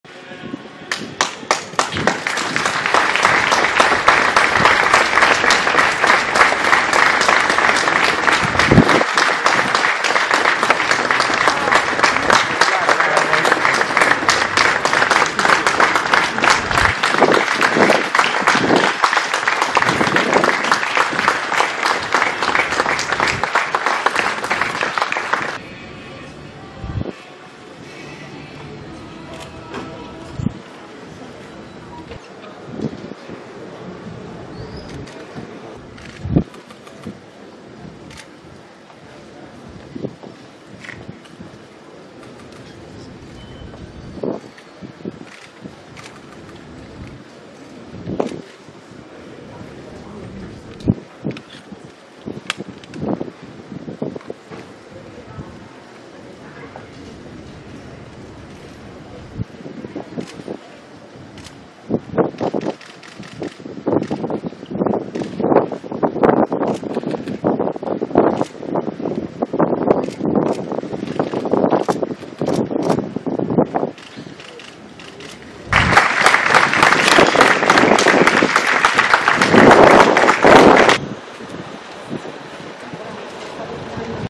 A Nusco l'ultimo saluto a De Mita: applausi all'uscita del feretro dalla chiesa
Presente anche il capo dello Stato Mattarella ai funerali dell'ex leader DC

(LaPresse) - Applausi all'uscita del feretro di De Mita, il leader Dc morto giovedì, al termine dei funerali durati poco più di un'ora a Nusco, nell'avellinese. All'uscita della chiesa il presidente Mattarella ha fatto le condoglianze alla famiglia per poi avvicinarsi al carro funebre per un ultimo saluto. ( LaPresse/AP - CorriereTv ). Guarda il video su Corriere